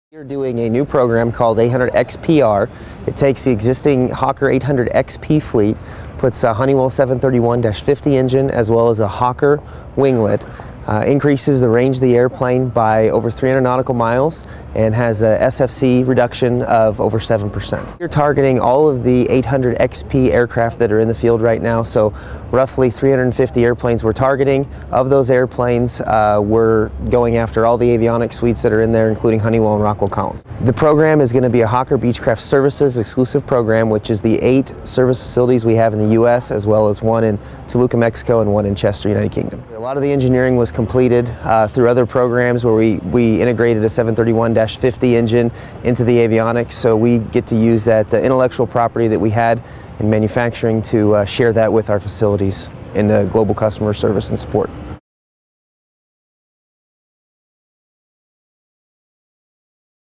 EBACE 2010: Hawker Beechcraft 800XPR
Hawker Beechcraft talk about their Dassault Falcon 800XPR upgrade package